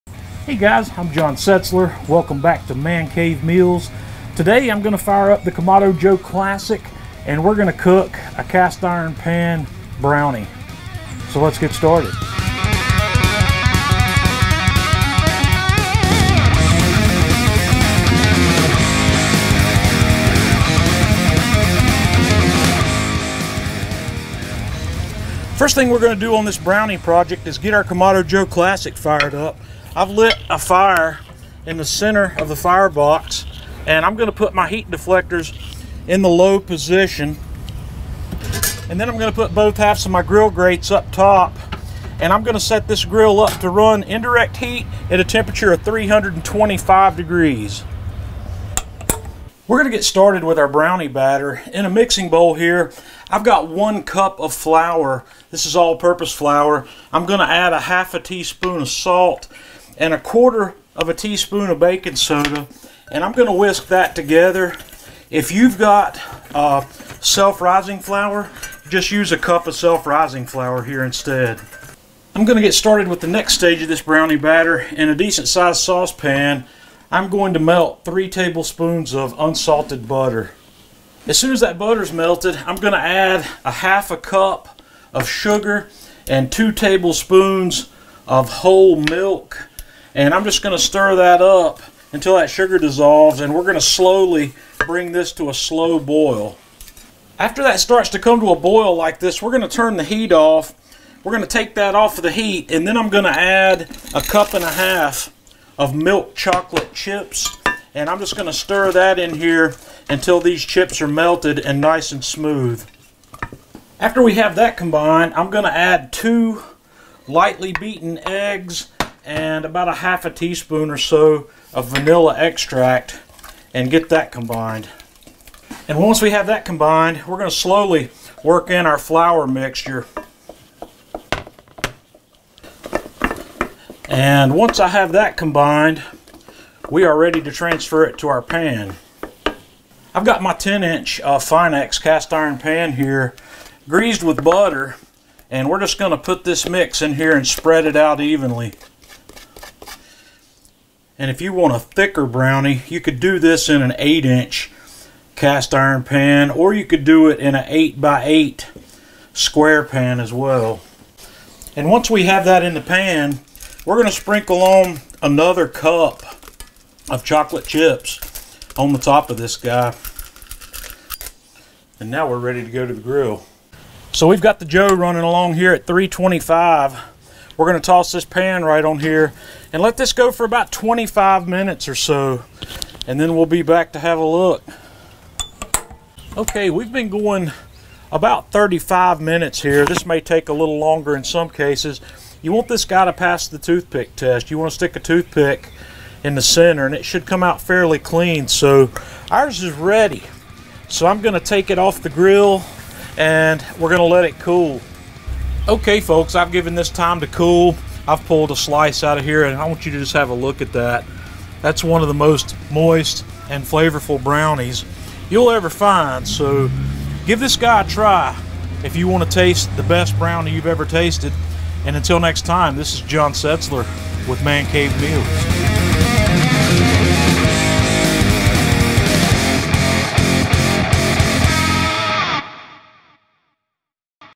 Iron Pan Brownie
I love simple baking on the Kamado Joe.  When you have a nice clean fire burning with the Kamado Joe Big Block charcoal, you can cook anything you like, including some seirous brownies!  Here's the recipe:

1 cup all purpose flour
1/4 tsp baking soda
1/2 tsp salt
1/2 cup granulated sugar
3 tablespoons unsalted butter
2 tablespoons whole milk
2 1/2 cups milk chocolate chips, divided
2 large eggs, lightly beaten
1/2 tsp vanilla extract

Preheat your grill to 325°F and grease a 10" baking pan or an 8x8" casserole with butter.

Combine flour, baking soda, and salt in a bowl and wisk together.  (If you have self-rising flour, just use that instead of making this mixture.)

In a saucepan, melt the butter then add the sugar and milk and stir until it comes to a light boil.  Remove from the heat and add 1 1/2 cups of the chocolate chips.  Stir until the chips have melted and are smooth.

Add the eggs and vanilla and stir until completely combined.

Add the flour mixture slowly and stir until completely combined.

Transfer the batter to your greased pan and top with remaining chocolate chips.  Place on the grill and cook until a toothpick inserted into the center pulls out clean (30-40 minutes.)

Let cool and enjoy!